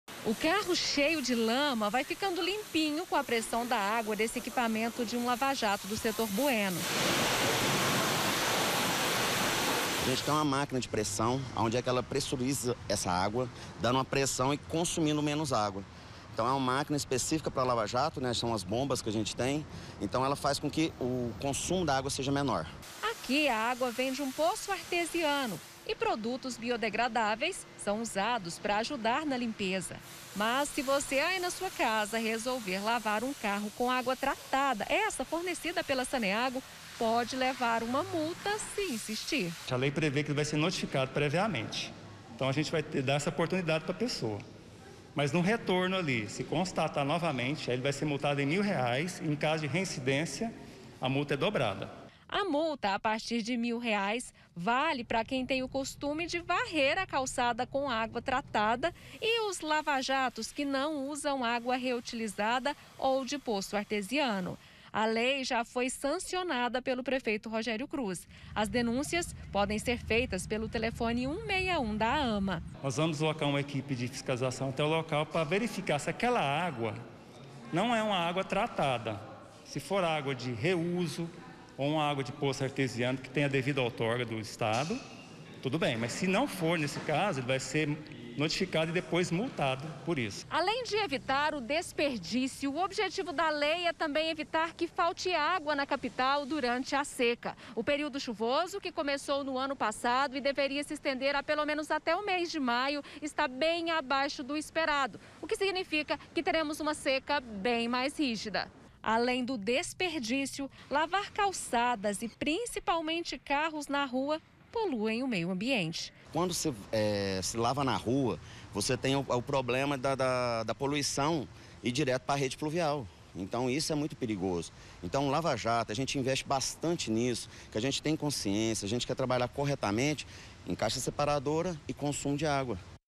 DESPERDÍCIO DE ÁGUA: LEI PREVÊ MULTA DE R$ 1 MIL PRA QUEM FOR FLAGRADO
JÁ ESTÁ VALENDO A LEI QUE MULTA QUEM FOR FLAGRADO DESPERDIÇANDO ÁGUA TRATADA EM GOIÂNIA.